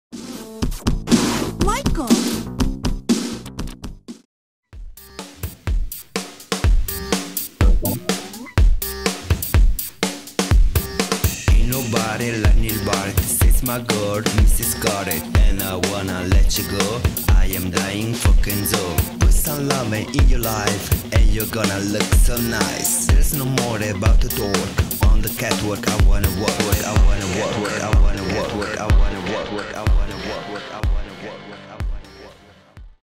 Waiting for AGDASH summer tour video backstage
Michael Massi TV presenta l'AGDASH summer tour visto da dietro le quinte...scherzi,giochi,shows,dancers,musica e altro ancora...

only on MICHAEL MASSI TV